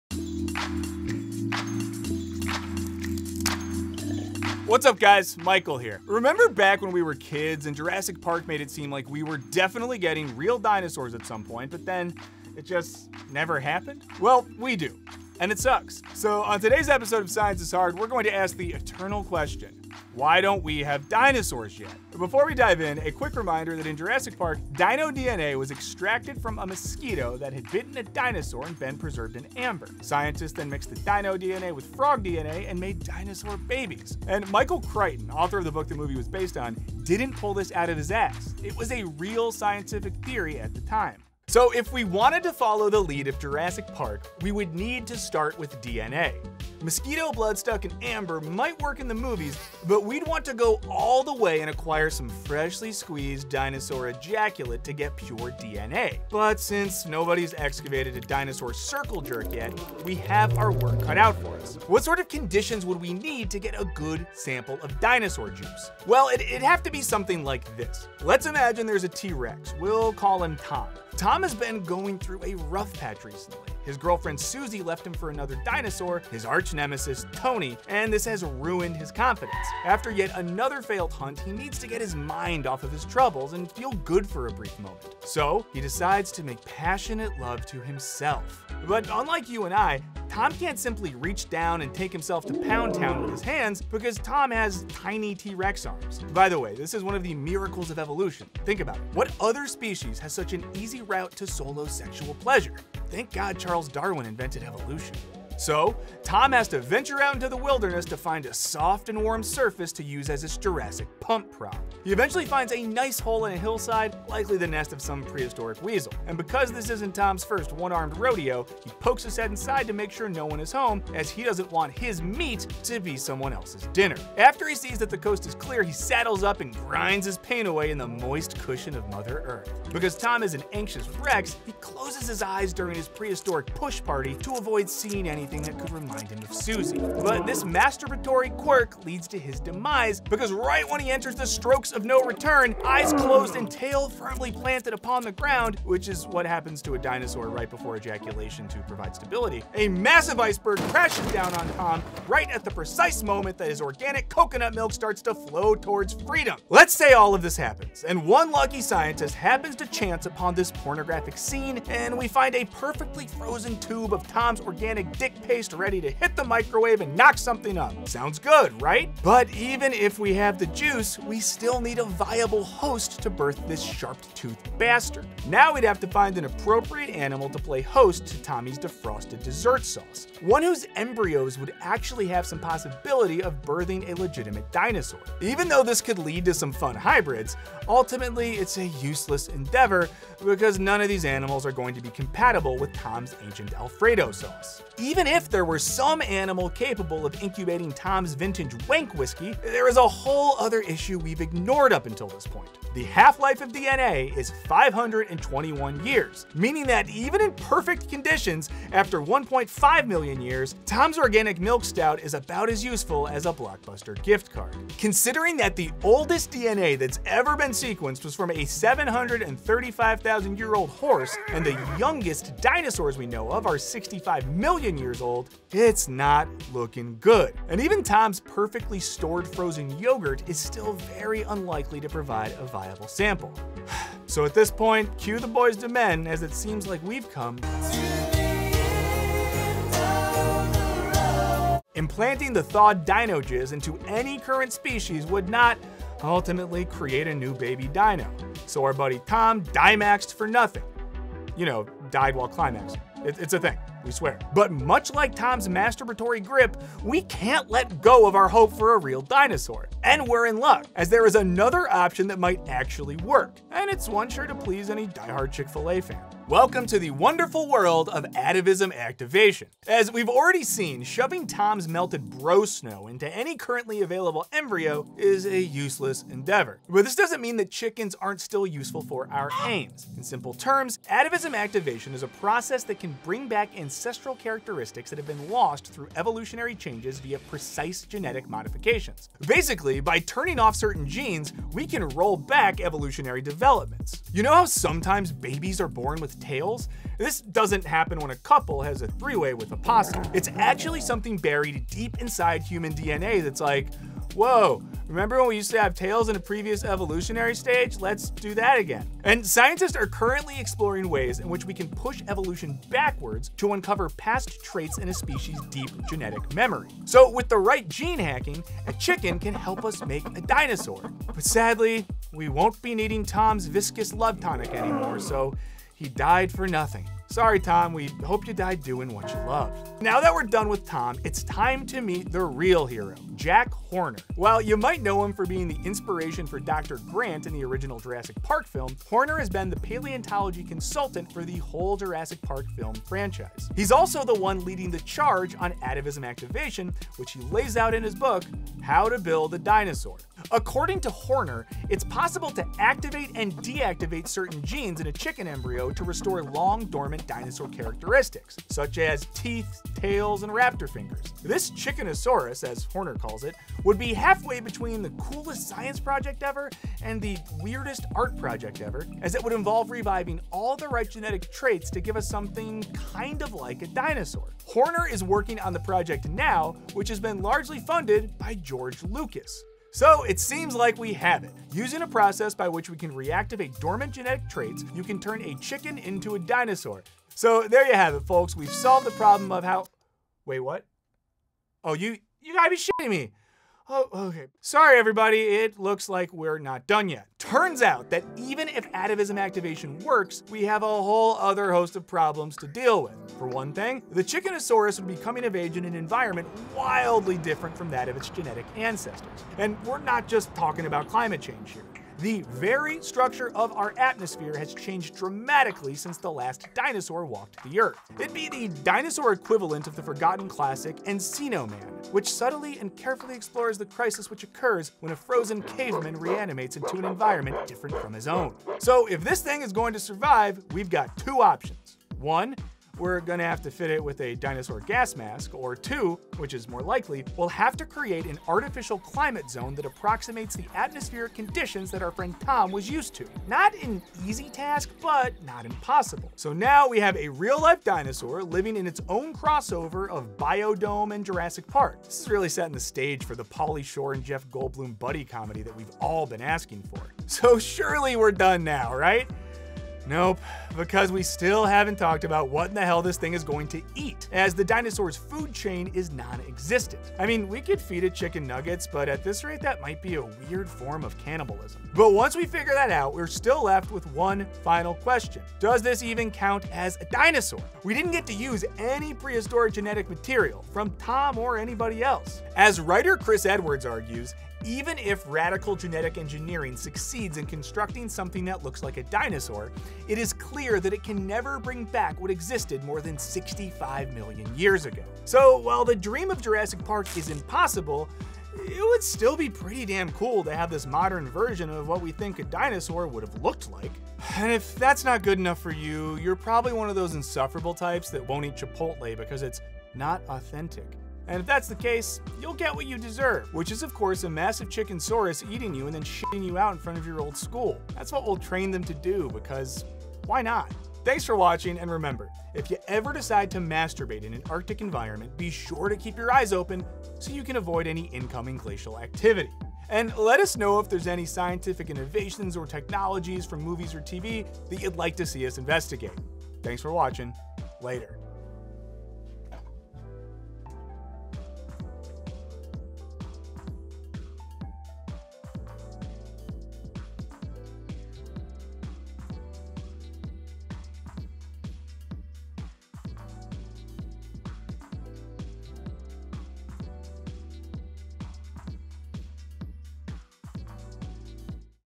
Can We Get a T-Rex from a Chicken? | Science is Hard
Will we ever get a real Jurassic Park?

We hope you like the first episode of our new series "Science is Hard". What concepts in science would you like to see us tackle in this new format?
Ever since we watched Jurassic Park as kids, we've all assumed that science would figure out how to make real dinosaurs. So what's the hold-up? Are we ever going to be able to have pet velociraptors? Let's find out on the first episode of our new series, Science is Hard.

Jurassic Park: A Perfect Movie?
Jurassic World: Fallen Kingdom – What Went Wrong?
Flat Earth: What Makes REAL Science?

00:00 - Intro
00:41 - Dino DNA
04:26 - Atavism Activation
07:53  - Conclusion

Research by Keith Comito
Written & Hosted by Michael Burns
Directed by Michael Luxemburg
Whiteboard Illustrations by Sean Godsey
Additional Art Assistance by Jocelyn Lee
Edited by Alex Lee
Video Title Card by Amanda Murphy
Produced by Evan Yee
Additional Production Assistance by Olivia Redden
Supervising Producers: Michael Burns & Amanda Scherker